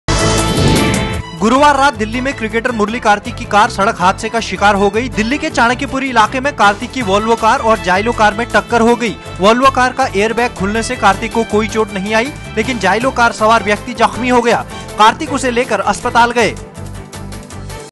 Cricketer Murali Kartik's car meets accident
Cricketer Murali Kartik's car meets accident. Kartik safe, but other car's driver injured.